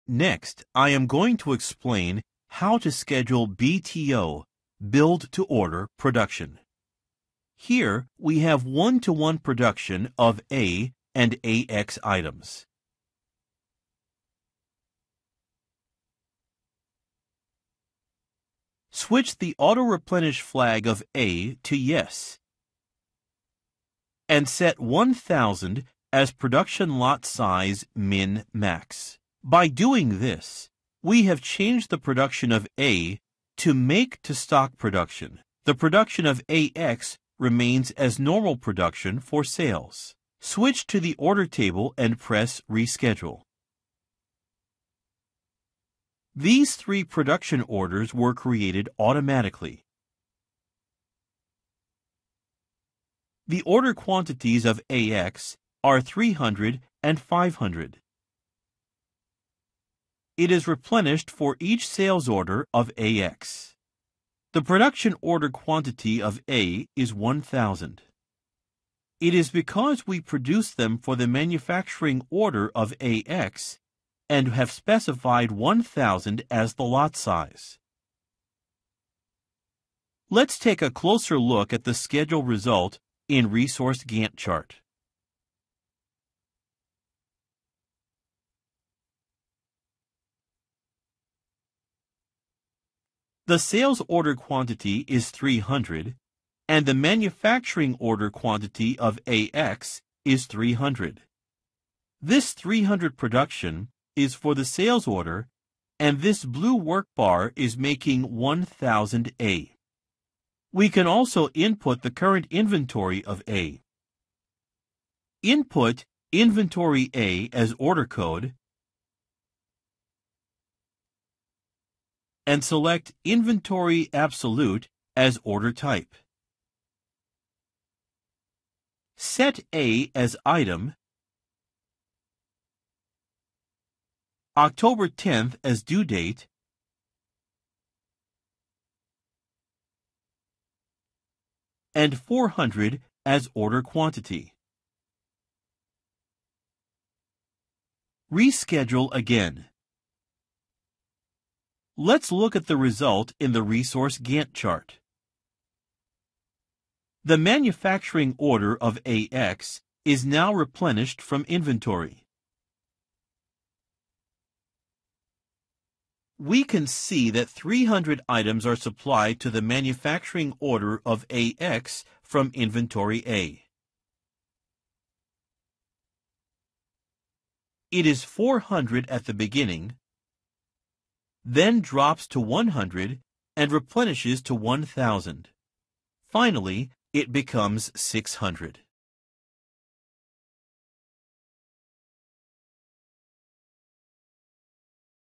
22.06 Planificación de Orden de Venta, BTO
A continuación, voy a explicar cómo planificar la producción BTO (Fabricar para Orden).
Aquí tenemos producción "uno-a-uno" de los artículos "A" y "AX".
Cambie la [Marca de auto-reposición] de "A" a "Sí",
y fije en 1000 el [Tamaño MAX/MIN de lote de Producción]
Haciendo esto, hemos cambiado la producción de "A" a producción "Fabricar para Almacenar"; la producción de "AX" se mantiene en la producción normal (para ventas).
Cambie a la [Tabla Orden] y pulse [Replanificar].
Estas tres órdenes de producción se crearon automáticamente.
Las cantidades de orden de "AX" son 300 y 500.
Está repuestao para cada Orden de Venta de "AX".
La cantidad de orden de producción de "A" es 1000.
Es debido a que los producimos para la orden de fabricación de "AX",
y hemos especificado 1000 como el tamaño de lote.
Veamos en detalle el resultado de la planificación en el [Gantt de Recursos].
La cantidad de orden de ventas es 300 y la cantidad de Orden de Fabricación de "AX" es 300.
Esta producción de 300 es para la orden de ventas y esta barra de trabajo azul fabrica 1000 de "A".
También podemos introducir el inventario actual de "A".
Introduzca "Inventario "A"" como [Código de Order]
y seleccione [Inventario (absoluto)] como Tipo de Orden.
Establezca "A" como [Artículo], "10 de Oct." como [Fecha de Entrega]
"400" como [Cantidad de Orden].
Replanifique de nuevo.
Miremos al resultado en el [Gantt de Órdenes].
La [Orden de Fabricación] de "AX" se repone ahora desde el Inventario.
Podemos ver que 300 artículos se proporcionan a la Orden de Fabricación de "AX" desde el Inventario "A".
Es 400 al principio, entonces cae hasta 100 y se repone hasta 1000; finalmente se convierte en 600.